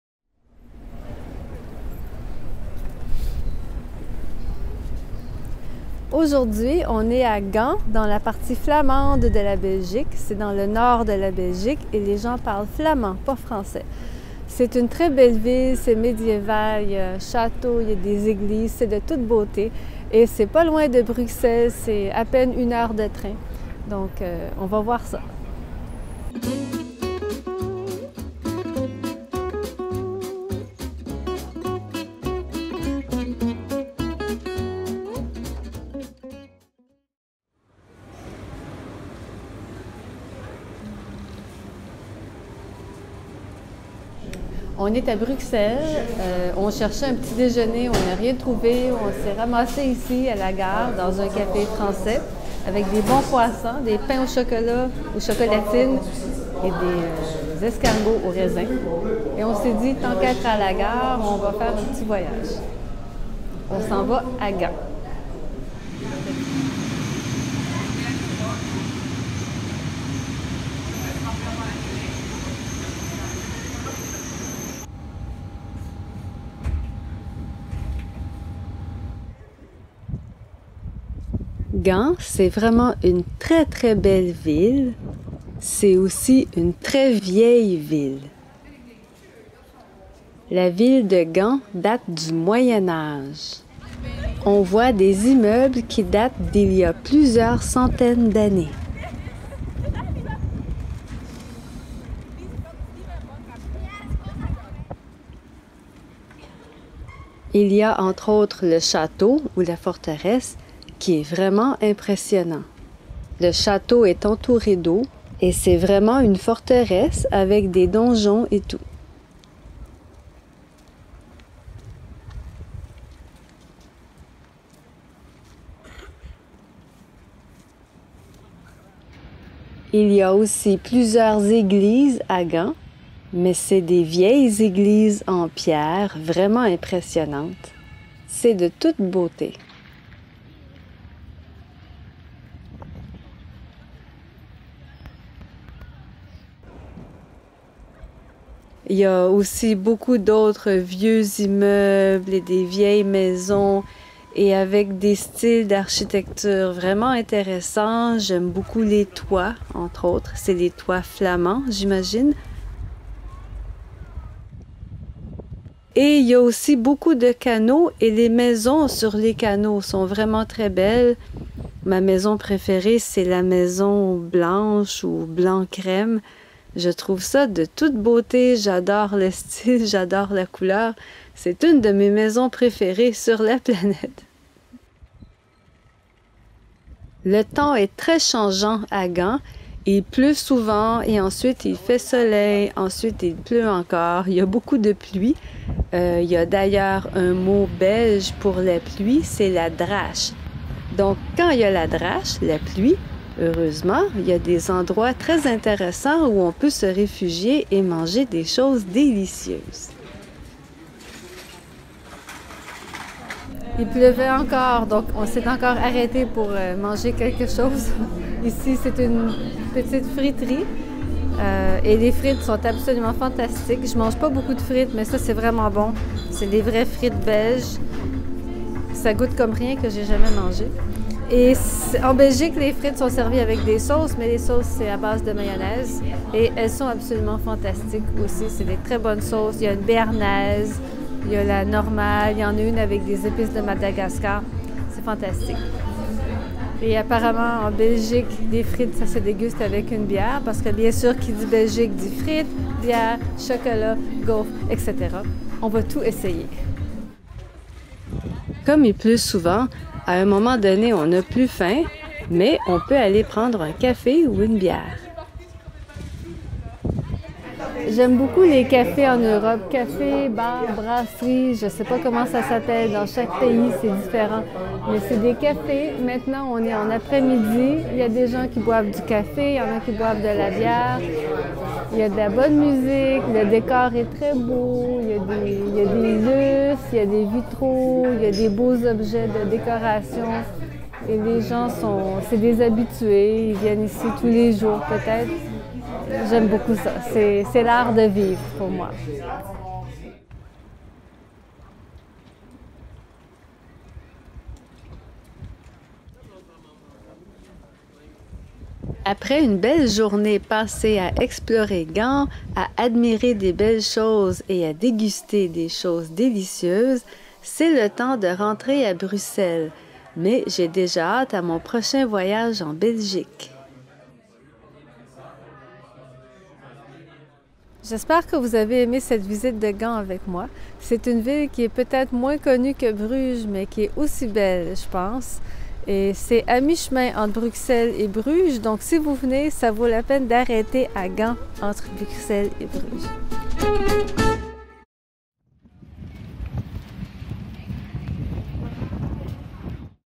Français facile: UNE PROMENADE À GAND EN BELGIQUE | Hélène se promène
Je vous fais découvrir Gand, une superbe ville médiévale belge.